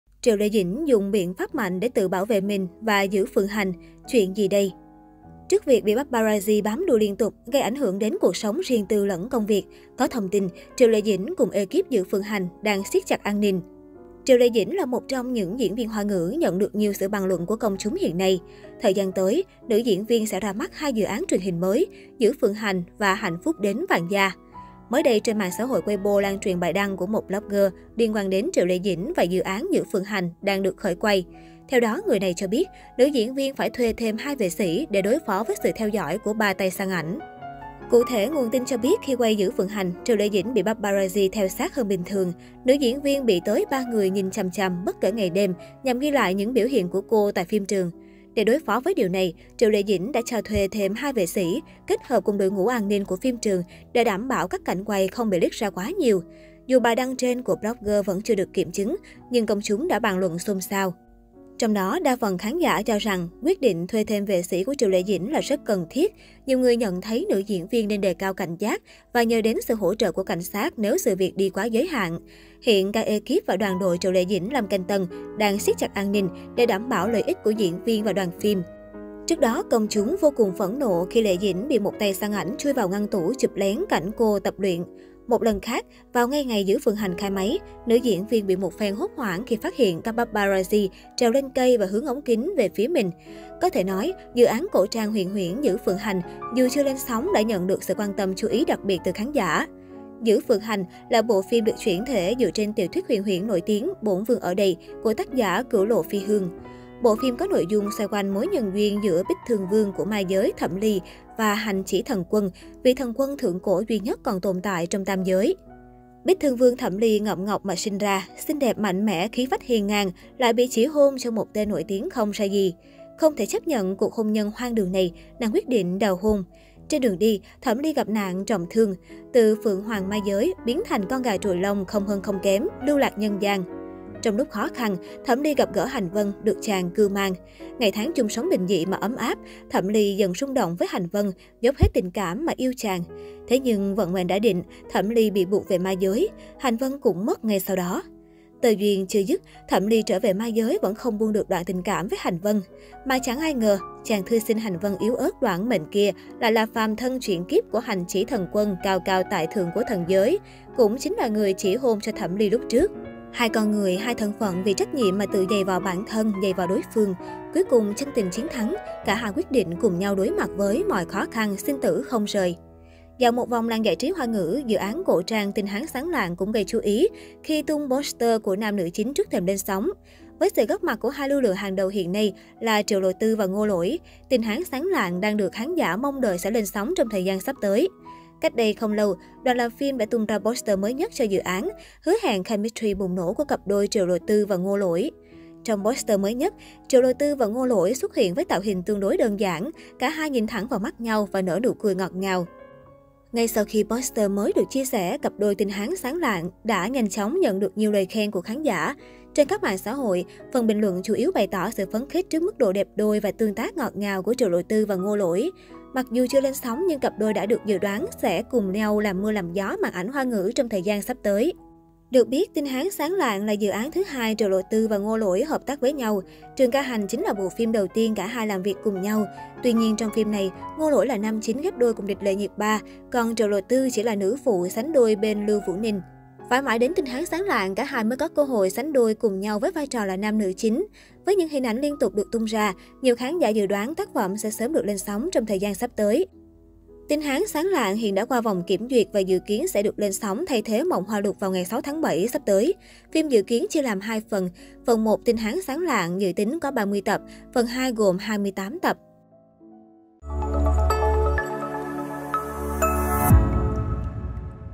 Triệu Lệ Dĩnh dùng biện pháp mạnh để tự bảo vệ mình và Dữ Phượng Hành, chuyện gì đây?
Triệu Lệ Dĩnh là một trong những diễn viên Hoa ngữ nhận được nhiều sự bàn luận của công chúng hiện nay. Thời gian tới, nữ diễn viên sẽ ra mắt hai dự án truyền hình mới - Dữ Phượng Hành và Hạnh Phúc Đến Vạn Gia.

Mới đây, trên mạng xã hội Weibo lan truyền bài đăng của một blogger liên quan đến Triệu Lệ Dĩnh và dự án Dữ Phượng Hành đang được khởi quay. Theo đó, người này cho biết nữ diễn viên phải thuê thêm 2 vệ sĩ để đối phó với sự theo dõi của 3 tay săn ảnh.